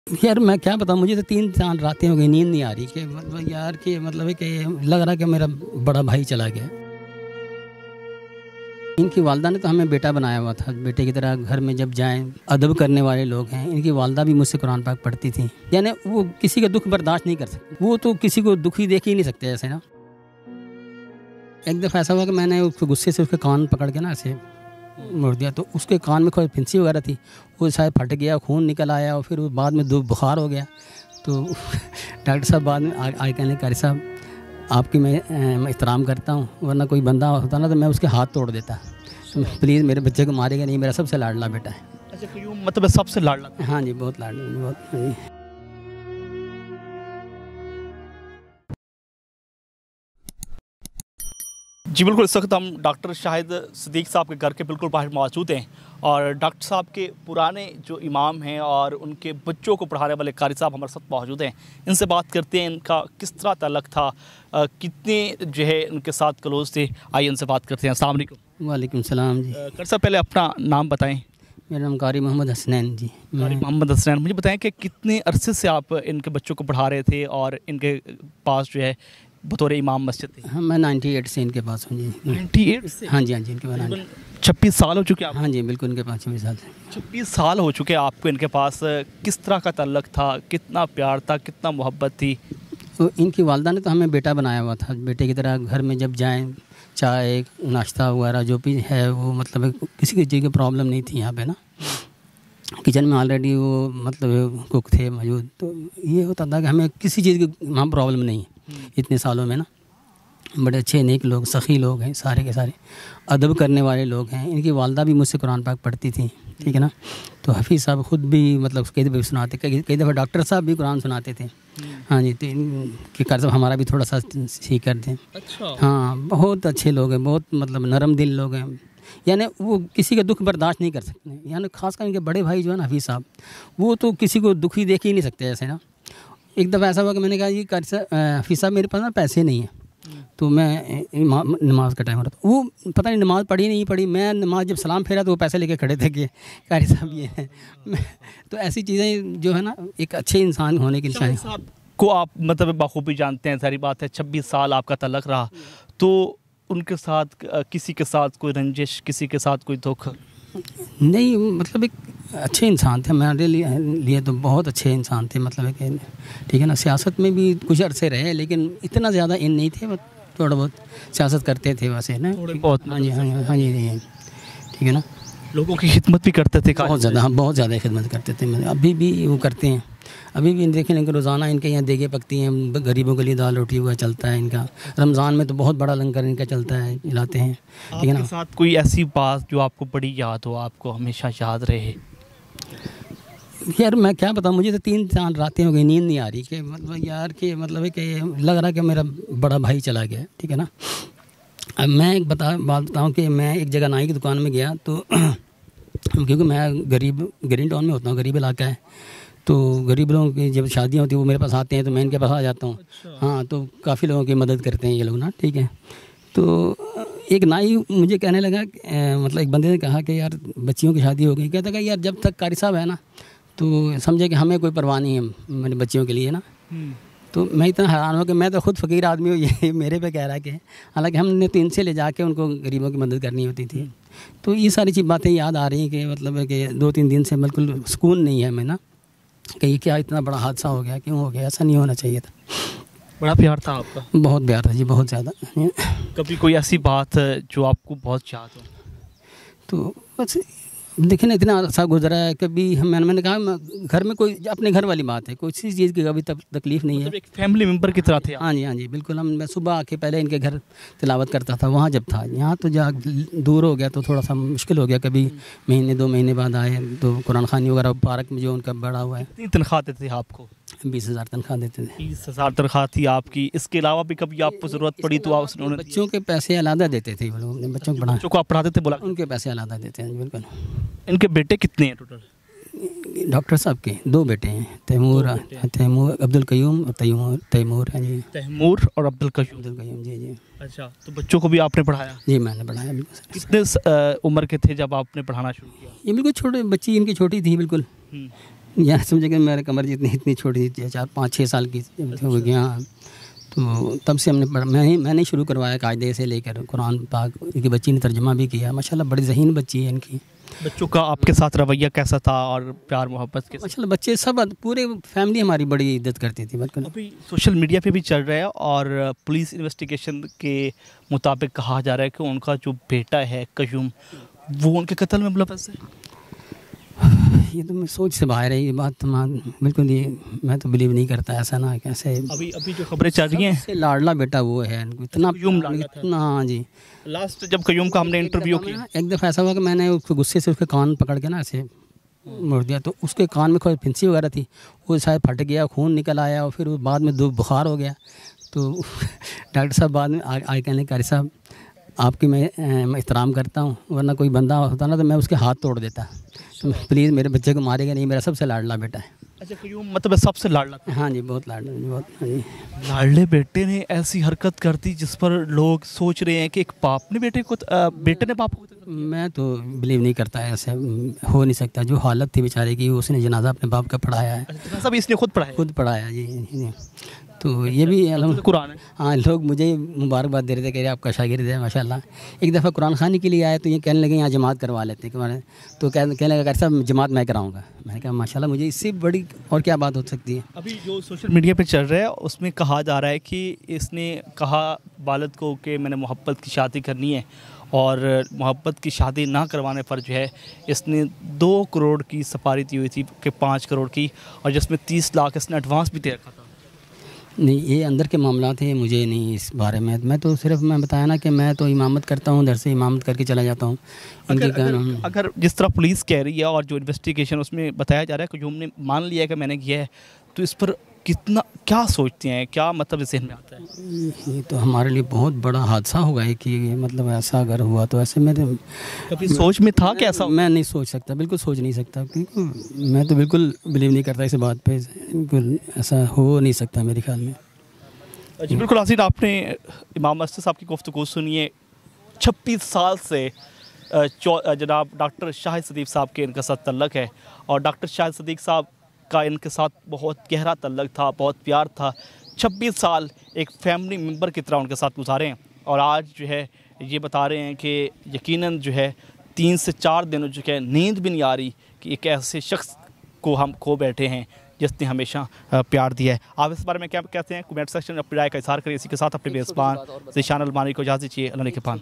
PTI Leader Dr Shahid Murder Case | Beta Qatil Nikla ? Pioneer World News
Host : Zeeshan Almani